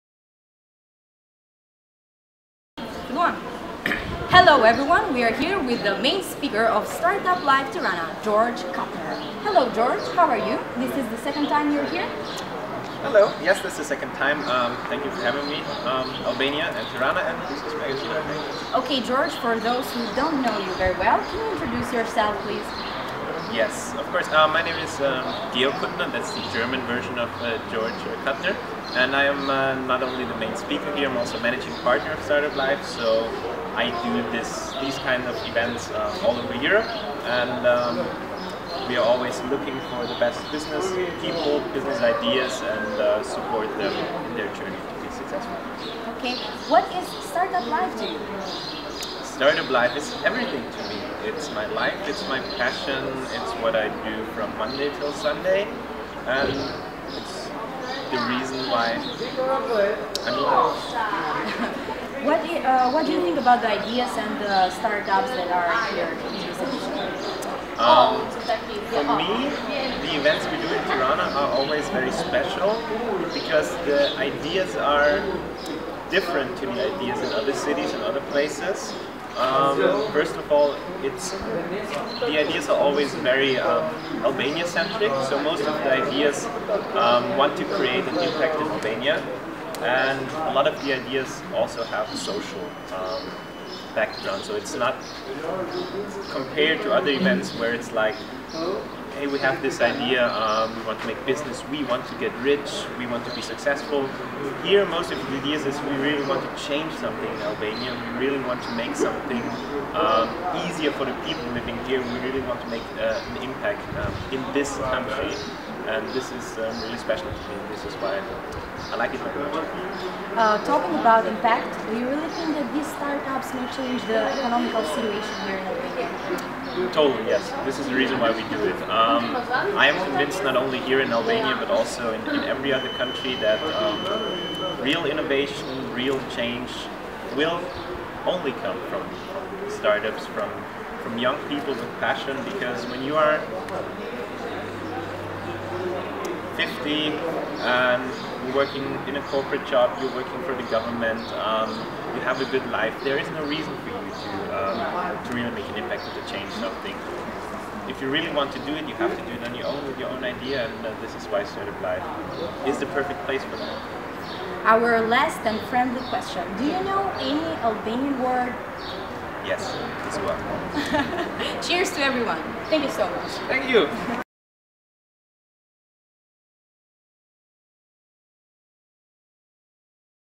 Startup Live Tirana 2017 - Intervistë me Georg Kuttner
Georg Kuttner është partner ndërkombëtar i Startup Live.
Startup Live Tirana është një event 3-ditor në të cilin të rinjtë konkurojnë me ide të ndryshme inovative të aplikueshme në fusha të ndryshme të biznesit.

Business Magazine pati kënaqësinë të ishte pjesë e Startup Live Tirana si media partner e këtij eventi.

Intervistë nga Neada Muçaj.